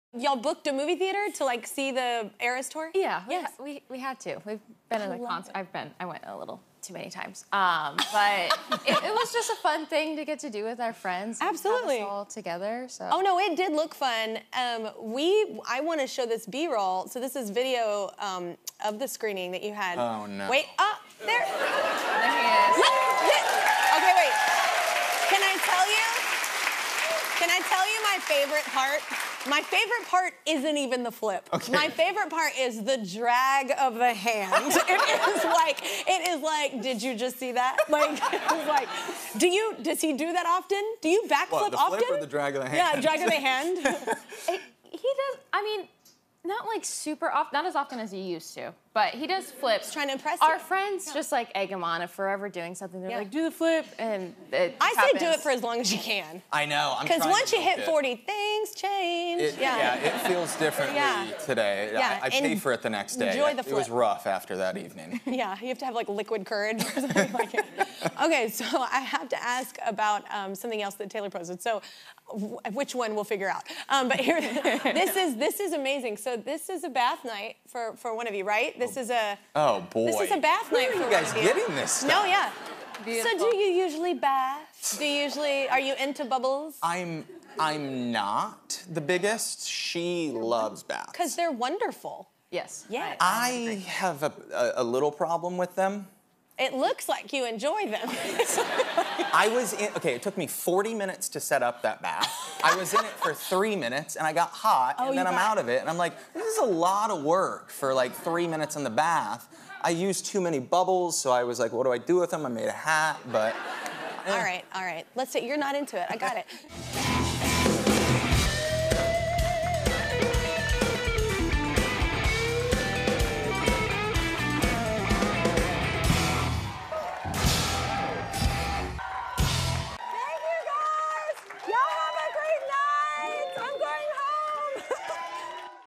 Taylor Lautner Backflipped Watching Taylor Swift's Eras Tour Movie
"It was rough after that evening." Taylor Lautner and his wife Taylor dish on renting out a theater to see "Taylor Swift: The Eras Tour" movie with their friends, and Taylor reacts to a video of himself doing a backflip during the film. Taylor also reacts to a hilarious picture of himself during a bubble bath that he had conflicting feelings about. Tune in today for more fun with the Taylors Lautner!